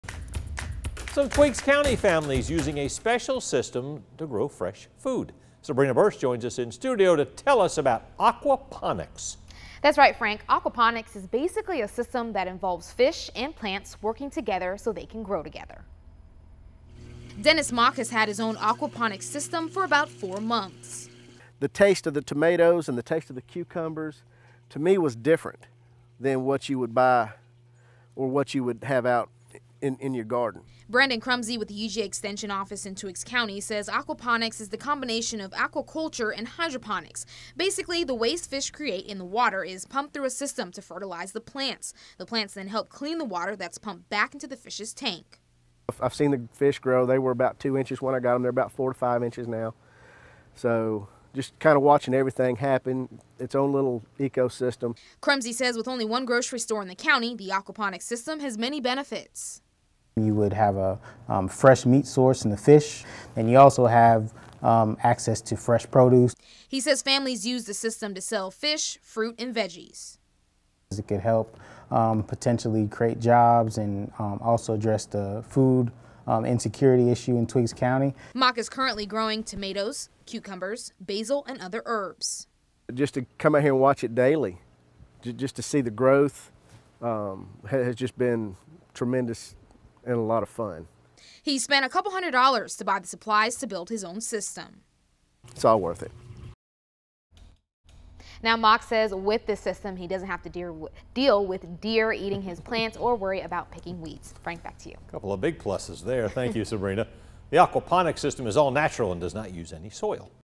Twiggs County families use aquaponics system to grow fresh food
Some Twiggs County families are using a special system to grow fresh food. Aquaponics is a system that involves fish and plants working together so they can grow together.

13WMAZ News is the #1 station for news and weather in the Central Georgia area, serving people in Dublin, Macon, Milledgeville and Warner Robins.